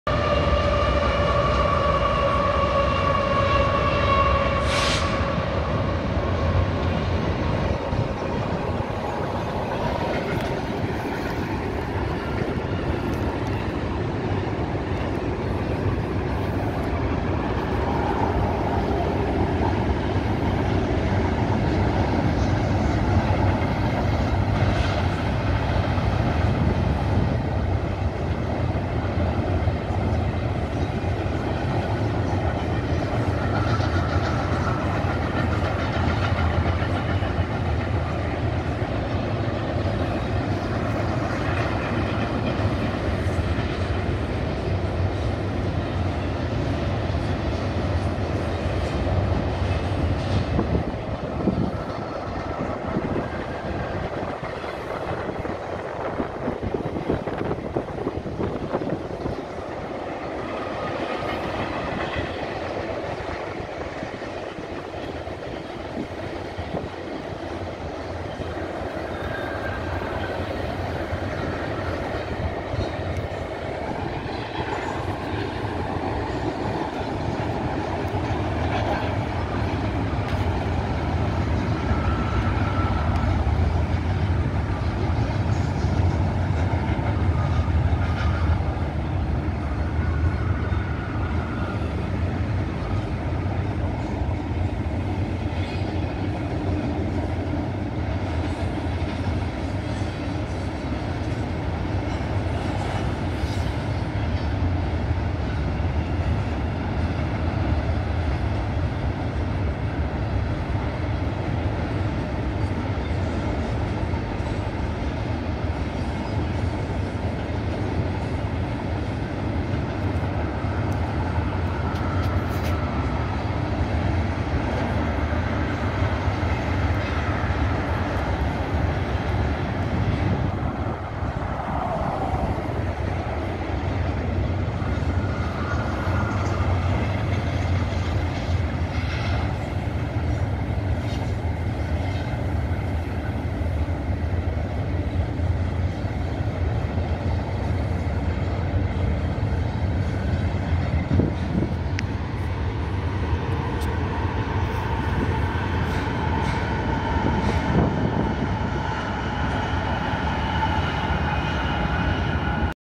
BNSF container train flimed from the old MP RR branch line.
BNSF container train north east bound to Kansas city. Running along the Kansas River. adjacent to the union pacific railroad line across the river.